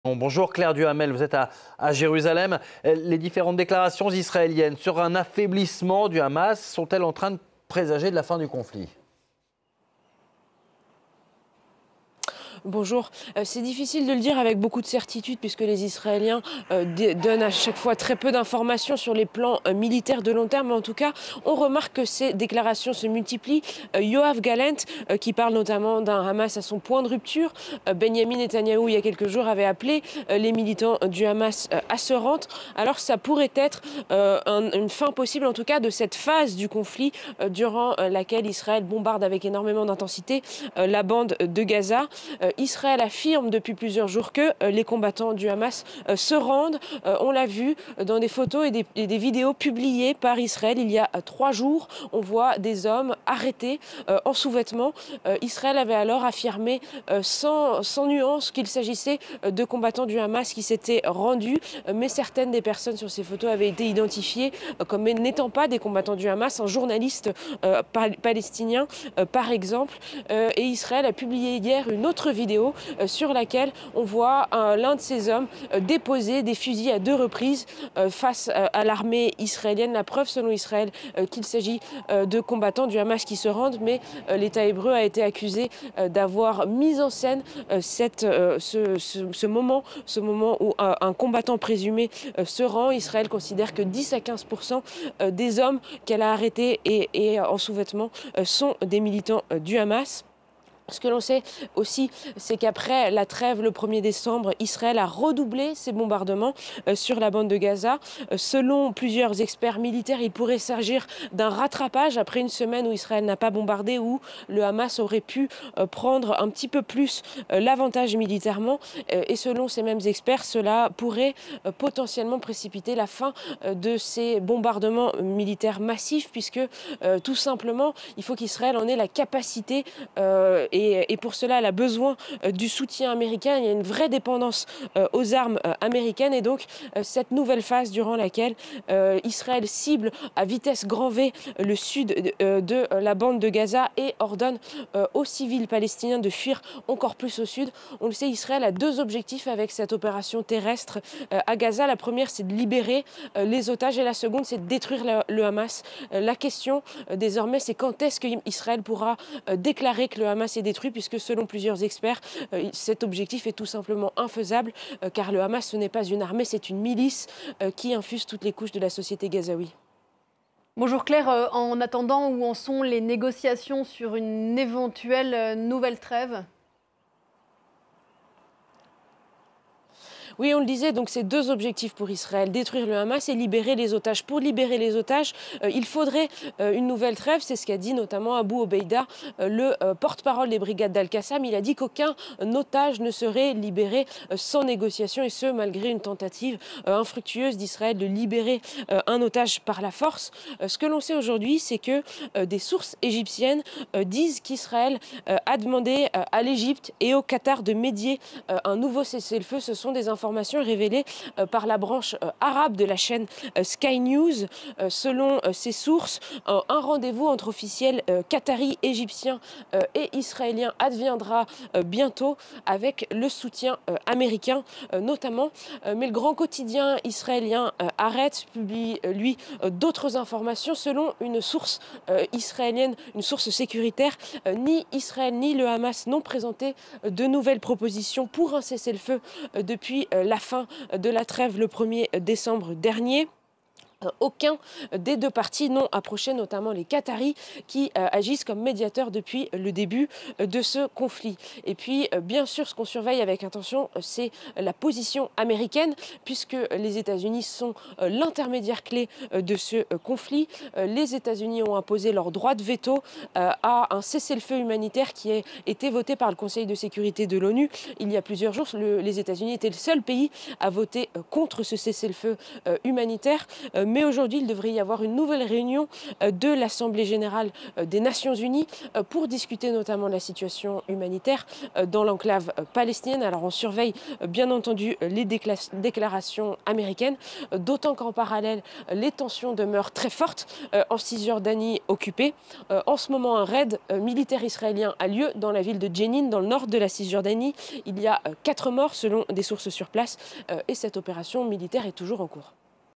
Gaza : vers une fin des bombardements israéliens massifs ? • FRANCE 24
Depuis la fin de la trêve à Gaza la 1er décembre, Israël a redoublé de forces pour bombarder le sud de la bande de Gaza. Mais pour certains experts, cela pourrait présager d'une fin prochaine de ces bombardements pour privilégier les combats au sol, faute de moyens et de munition, Israël étant très dépendant des Etats-Unis. Les précisions de Claire Duhamel, correspondante de France 24 à Jérusalem.